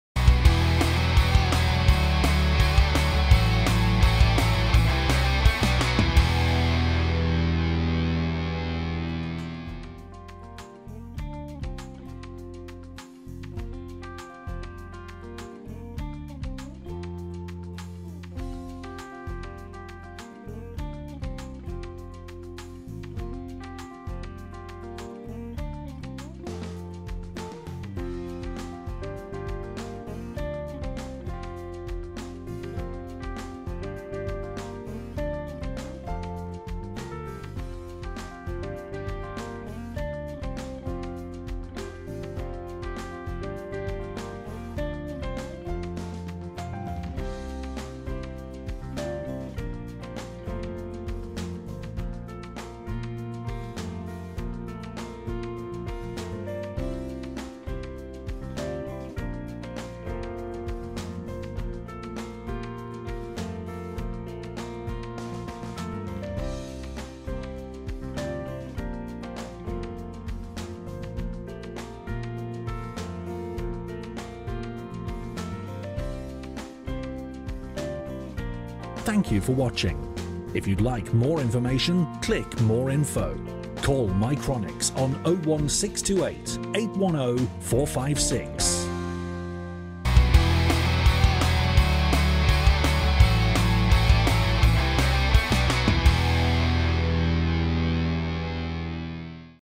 Ultrasonic Flow Meters - Hydroelectric Power Generation
Hydroelectric Power Generation is still a very important part of energy development in this day and age. Ultrasonic Flow Meters are used regularly in this process to help measure the water flow in the Industry. This excellent video will provide you with more information on the use of Ultrasonic Flow Meters in Hydroelectric Power Generation.